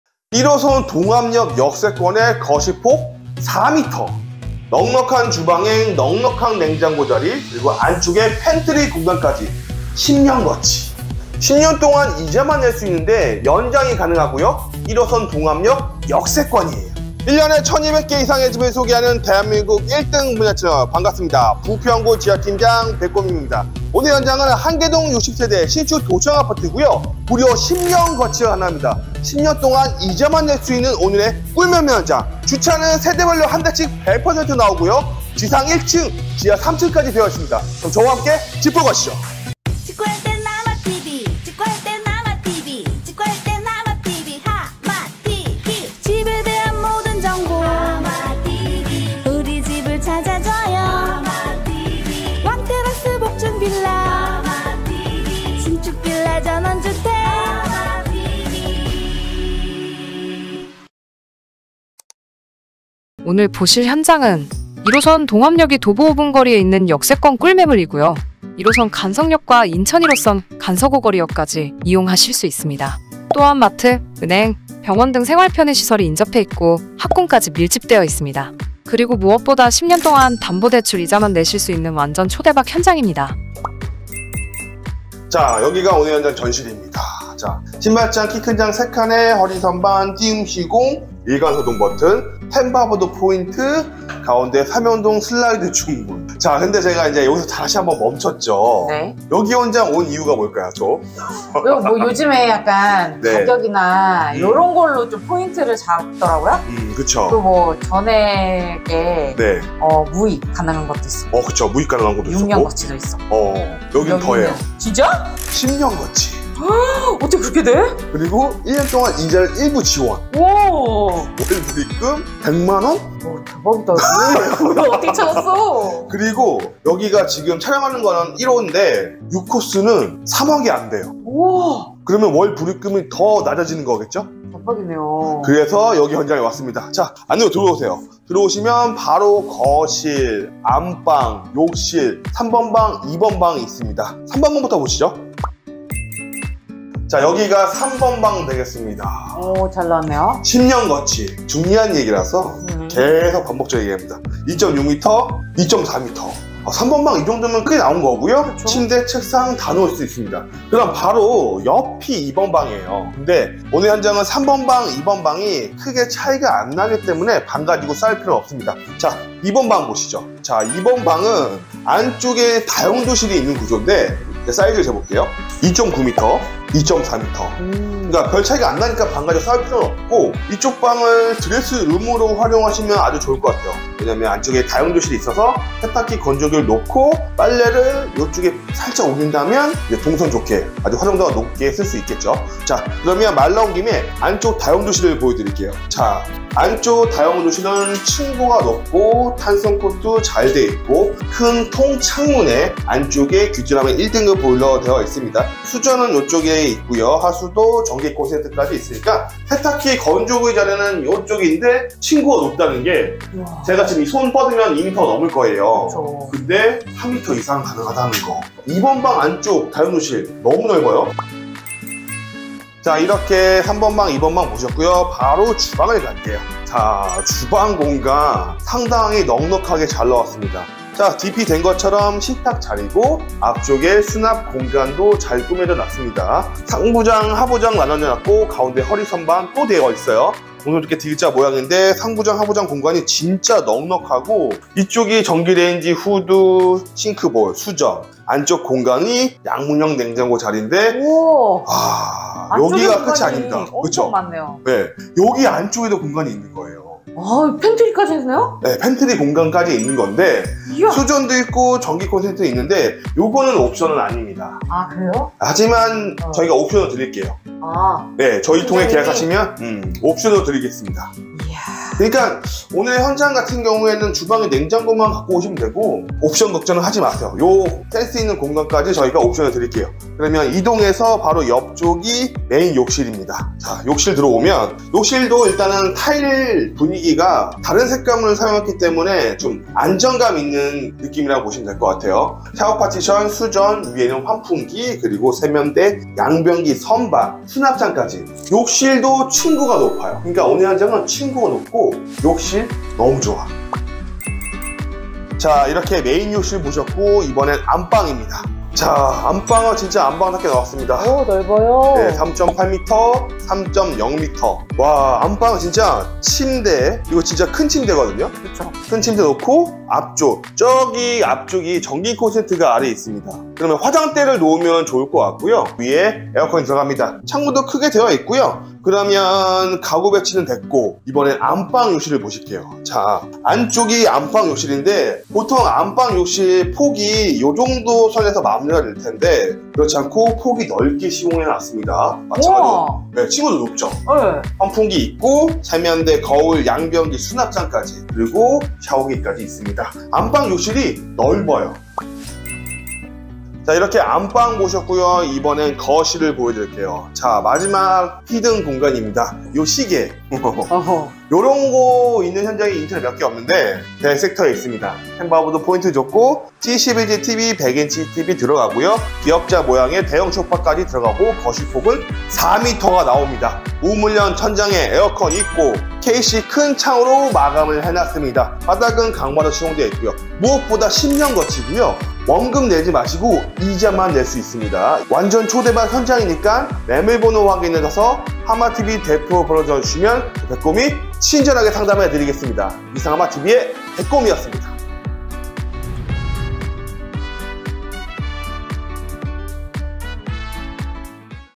[인천신축빌라] 10년 동안 이자만 내고 살아요~ 부담없지요? 내가 사는 동안 부담없는 부평구 신축빌라! /4868
★ 현장 정보
- 부평구 십정동
- 1개동 10층 63세대
- 3룸(방3개, 화2개), 실평수 23평
- 시스템에어컨 2대 옵션(거실, 안방)
- 주차 100%

★ 인프라
- 1호선 동암역 도보 5분
- 인천1호선 간석오거리역 도보 8분
- 간석자유시장, 근린공원 인접
- CGV, 롯데백화점, 롯데시네마, 이마트, 홈플러스 인접

★ 학군
- 동암초중, 약산초, 간석초중, 간석여고 상인천초중, 신명여고, 인제고

★ 병원
- 가천대 길병원

분양가
3룸 3억초중반~
실입주금 6천만원~
10~15년 담보대출 이자 거치 가능
1년동안 1.5% 이자 지원